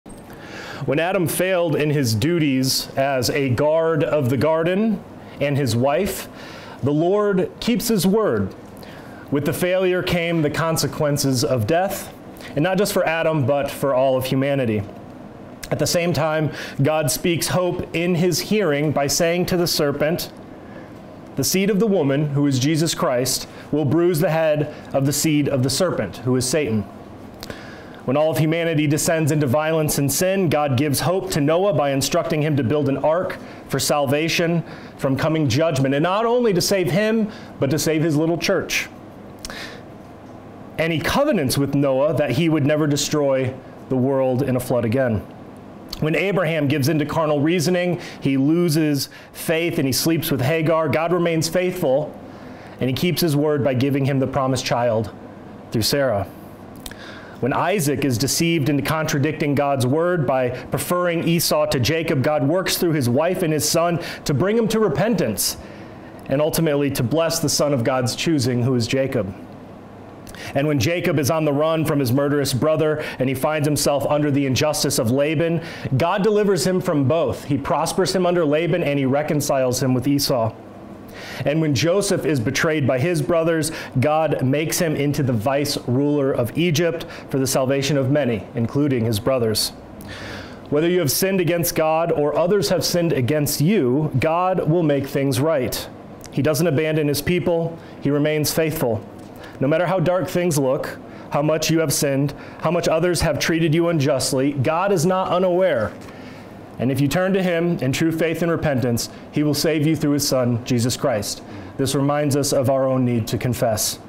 Genesis and Hope (The Gospel in Genesis)
Confession exhortation at Divine Service by Fr. Jack Shannon at Saint Athanasius Church, September, 29, 2024 AD.

Contra Mundum Swagger